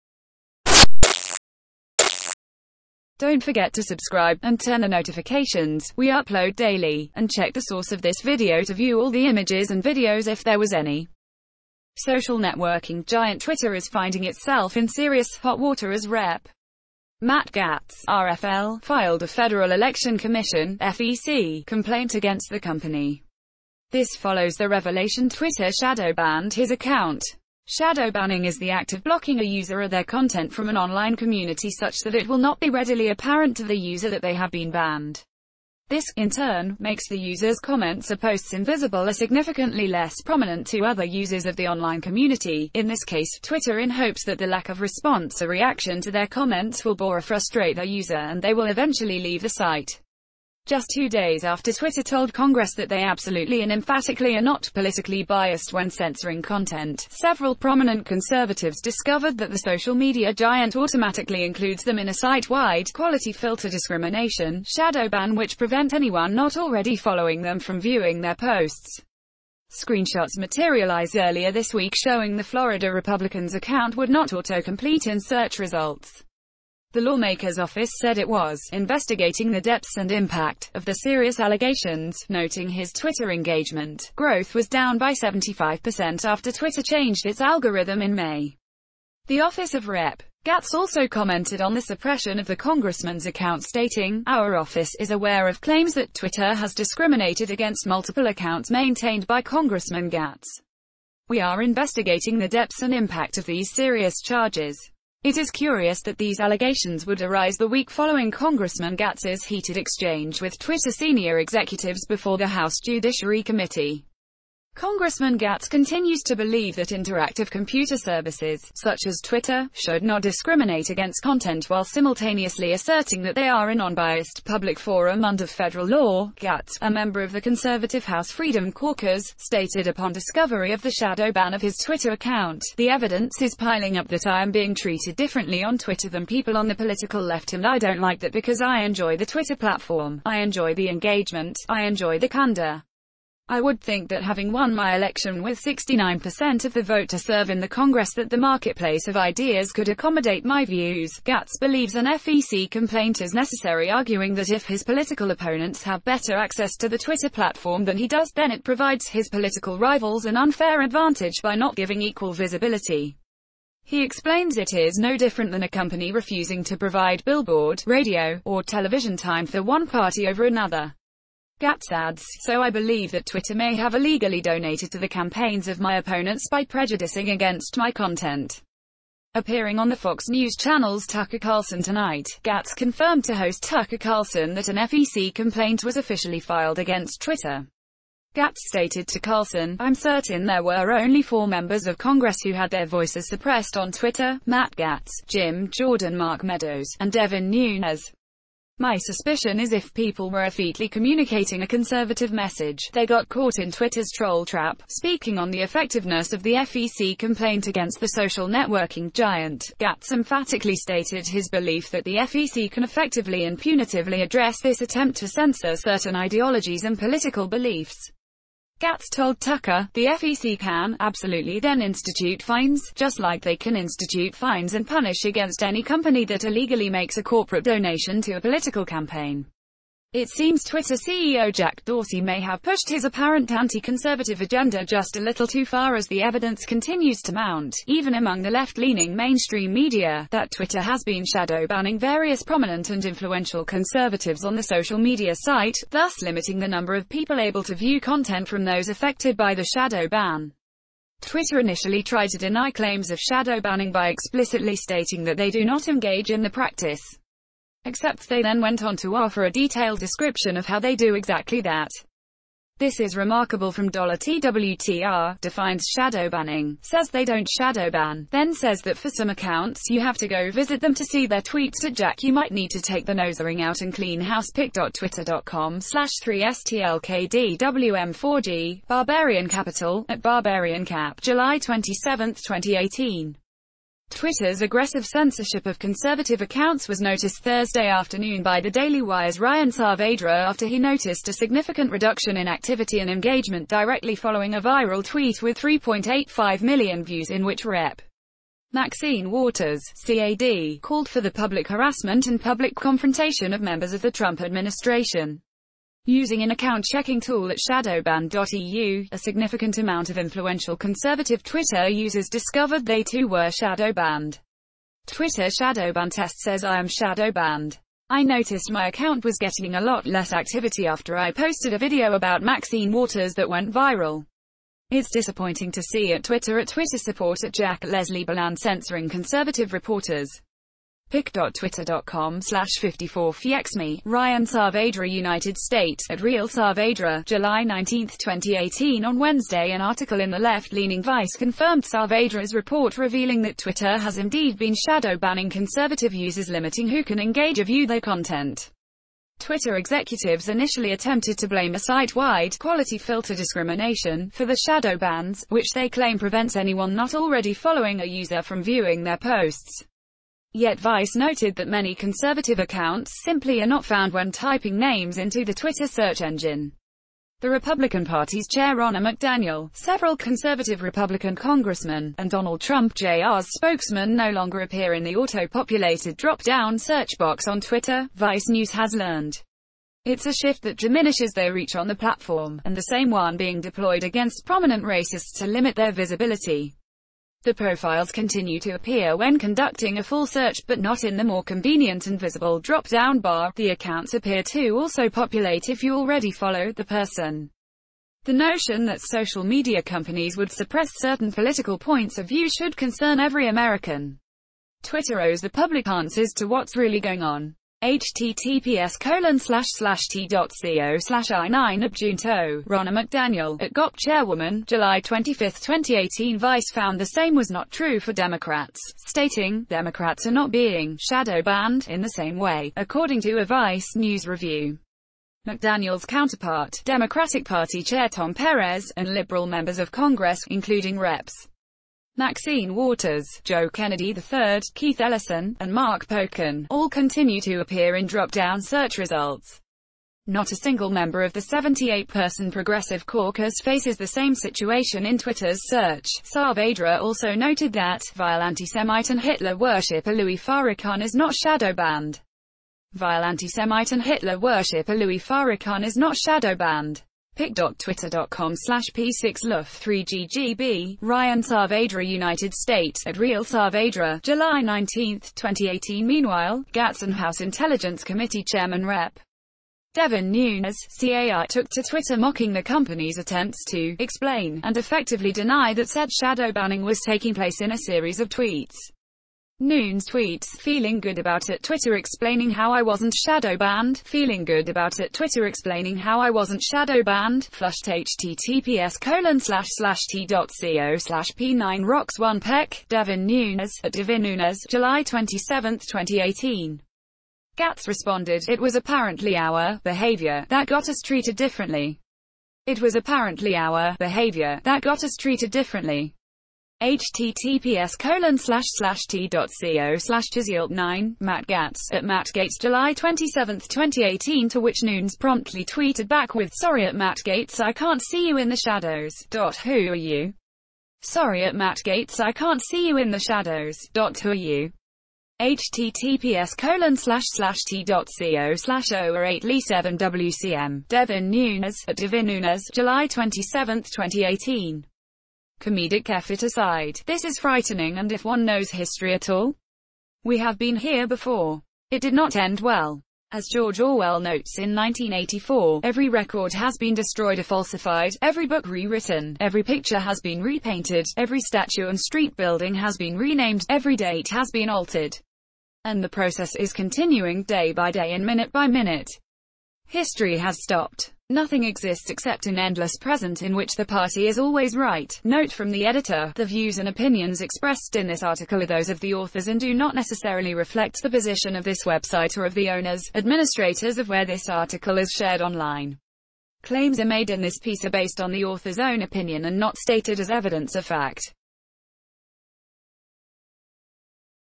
BREAKING: Rep. Matt Gaetz Filing Federal Complaint Against Twitter After He Caught Them Red–Handed
Source DONT' FORGET TO SUBSCIBE TO OUR CHANNEL Social networking giant Twitter is finding itself in serious hot water as Rep. Matt Gaetz (R-FL) filed a Federal Election Commission (FEC) complaint against the company. This follows the revelation Twitter shadow banned his account.

Shadow banning is the act of blocking a user or their content from an online community such that it will not be readily apparent to the user that they have been banne ...